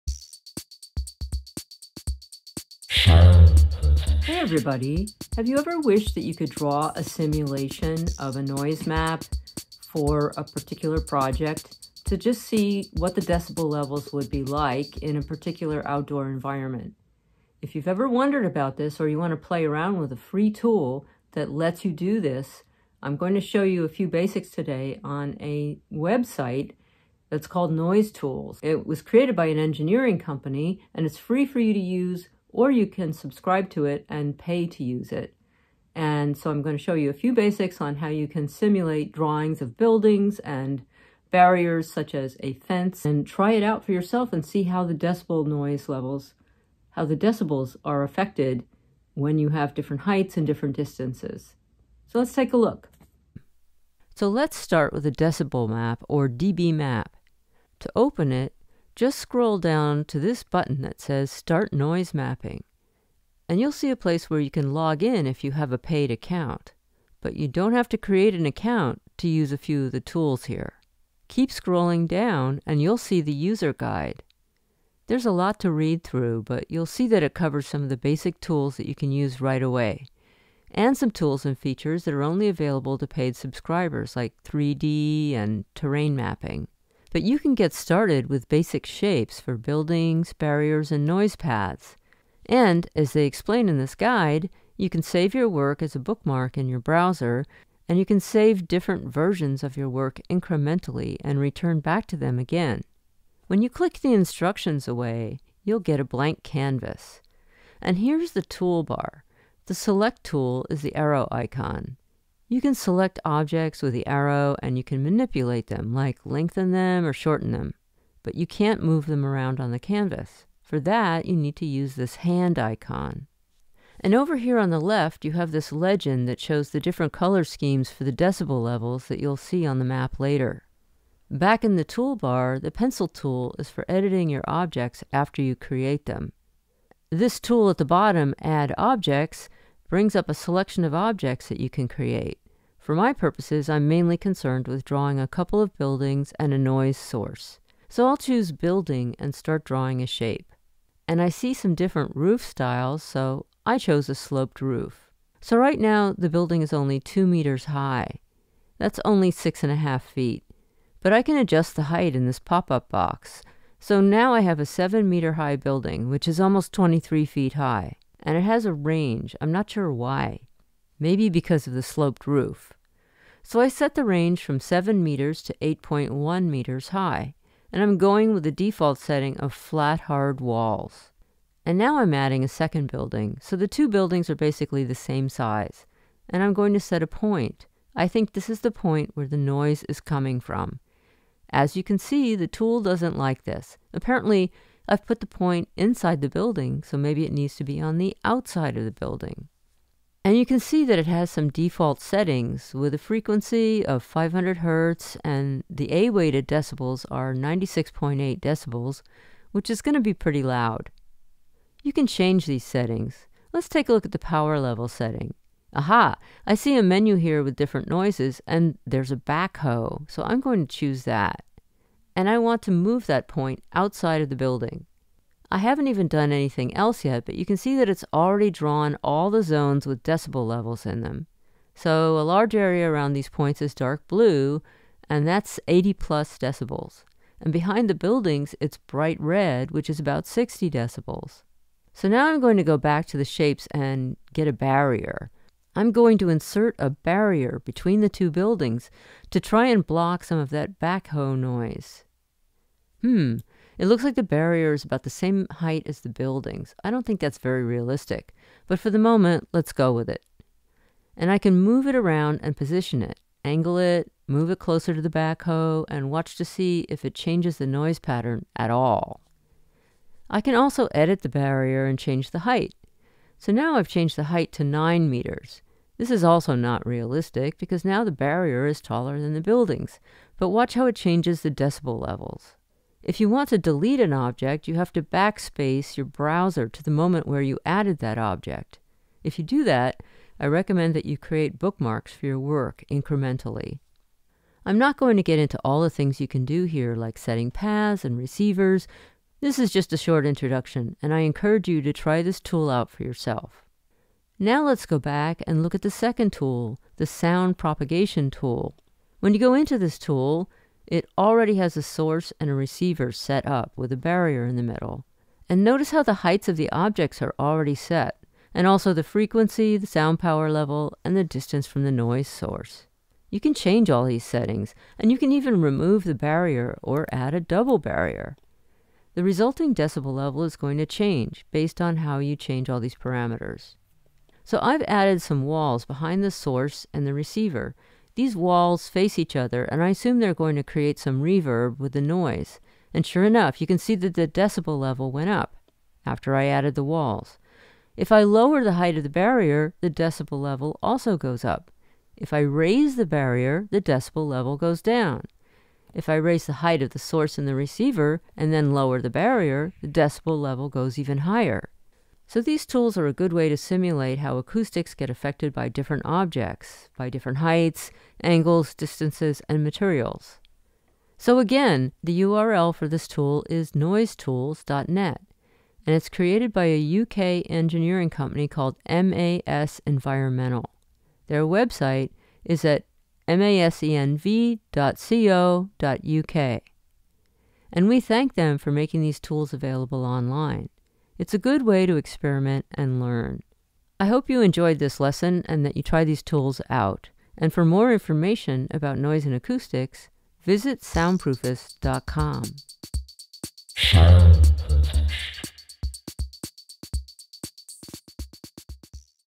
How to create your own noisemap
Noise Tools is a free online noisemap and decibel-level simulator from MAS Environmental (an engineering firm in the UK). You can simulate the decibel levels in different scenarios by creating buildings, walls, and barriers, and then change their height, angle, distance, terrain, or noise sources.  The online simulator comes with a few simple shapes that you can draw, resize, and move around on a canvas. As you move the objects or change the noise source, you'll see how the decibel level rises and falls.

This is just a quick demo to show the potential of this tool. The engineering company that created it also made some demo videos: